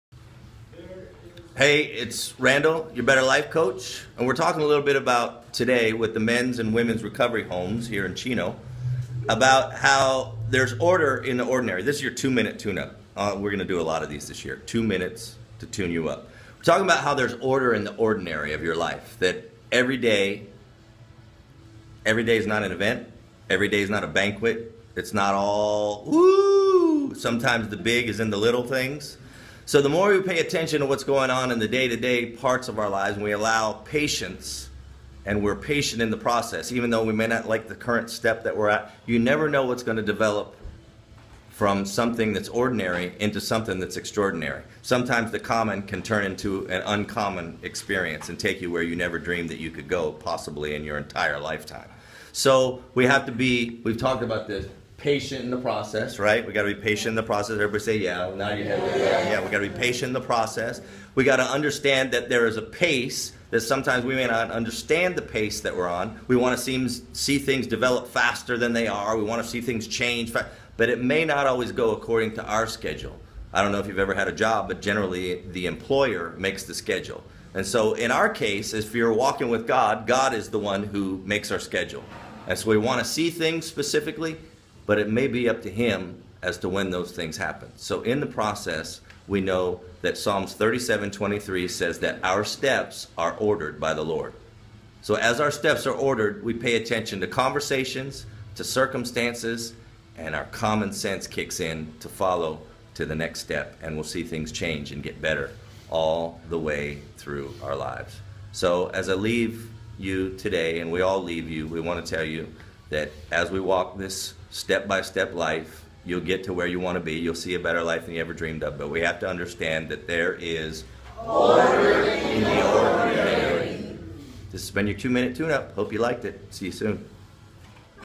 2 Minute TuneUp : Order in the Ordinary
Sometimes the every day routine is just so...mundane. The BetterLife Coach wants to highlight how no matter how ordinary your day in and day out may be, there's likely an order to your ordinary.